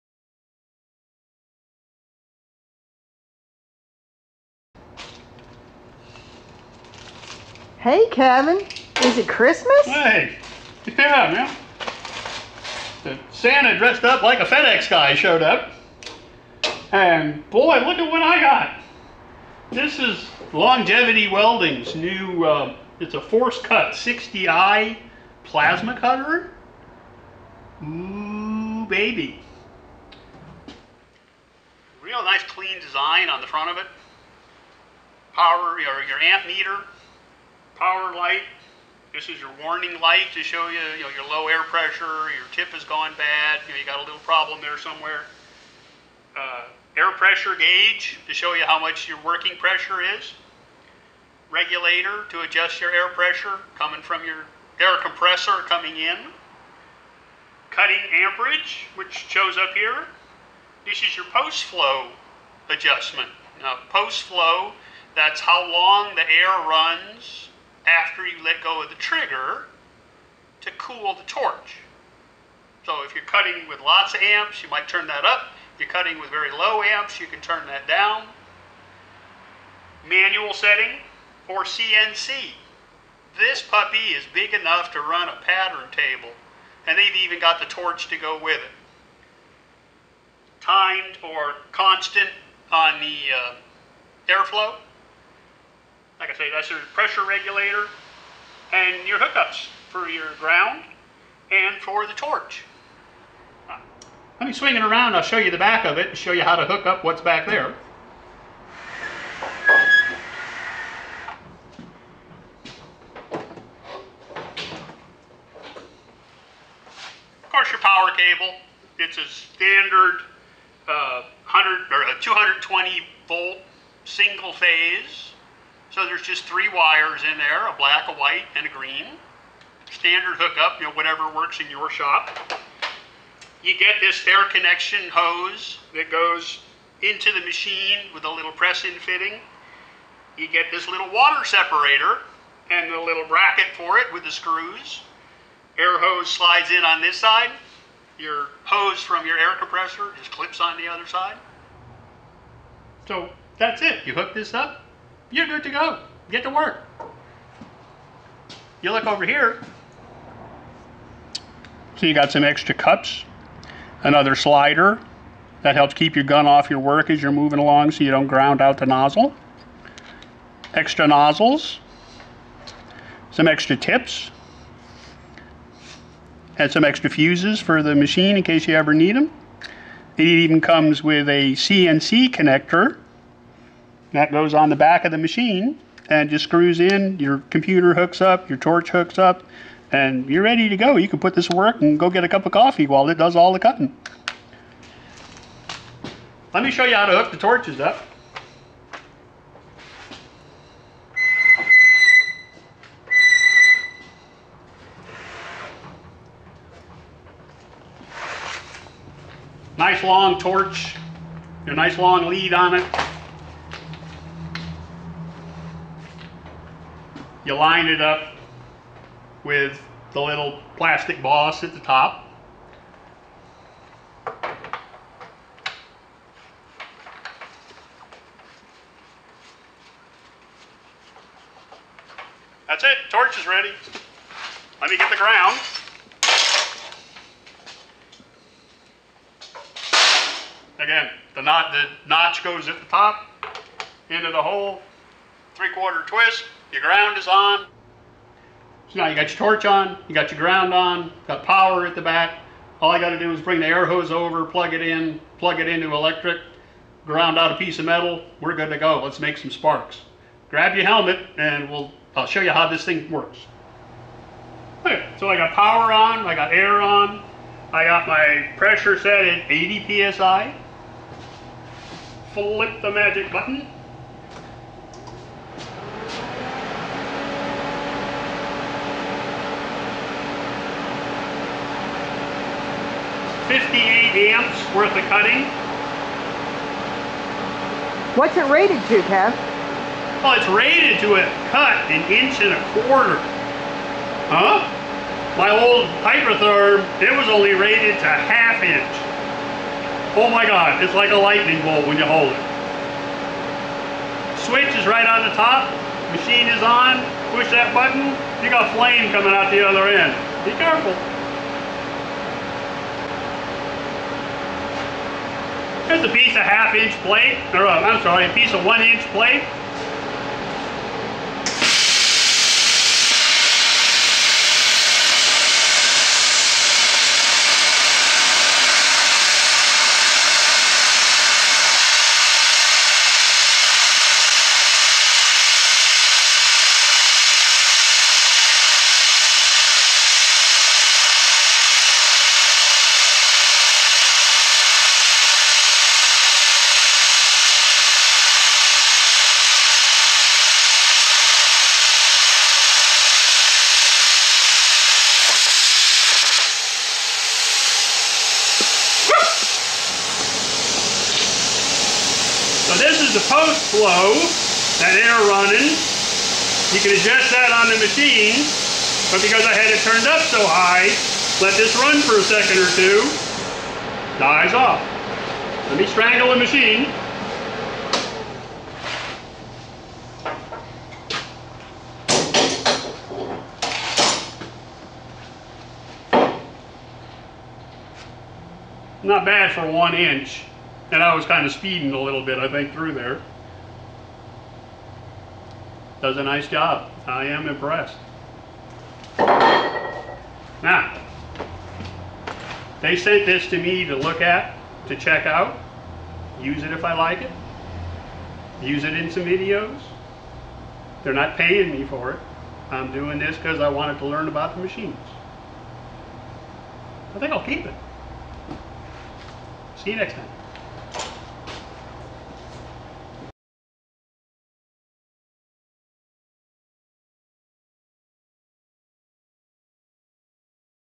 How to Set Up a Longevity Plasma Cutter - Kevin Caron
- Artist Kevin Caron shows the parts of the Longevity ForceCut 60i Plasma Cutter and how to set it up. Then he cuts a 1-inch plate in 35 seconds - wow! At $900, this plasma cutter will cut up to 1-1/4", while the Hypertherm Kevin has been using, which cost $1500 several years ago, is only rated to 1/2". To learn more about the Longevity ForceCut 60i,